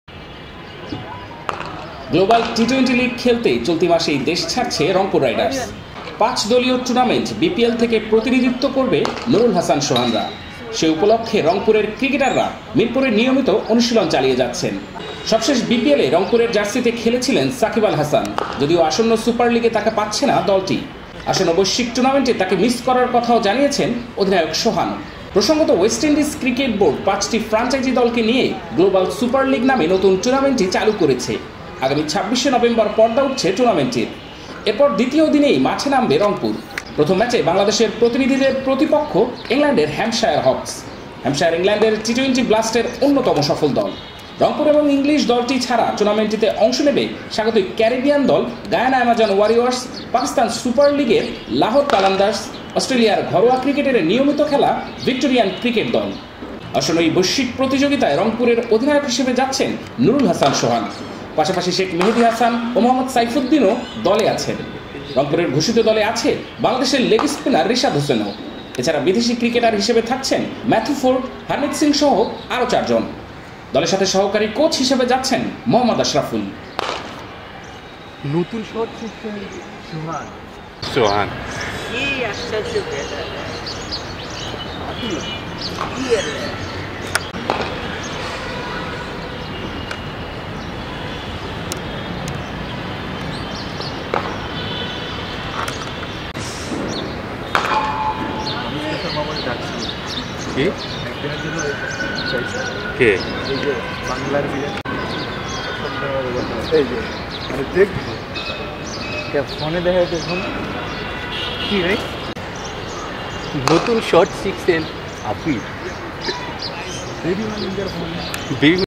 গ্লোবাল টি-টোয়েন্টি লিগ খেলতে দেশ ছাড়ার আগে রংপুর রাইডার্স অনুশীলনে। Rangpur Riders
গ্লোবাল টি-টোয়েন্টি লিগ খেলতে দেশ ছাড়ার আগে রংপুর রাইডার্স অনুশীলনে।
See less

Address:
Cricket Today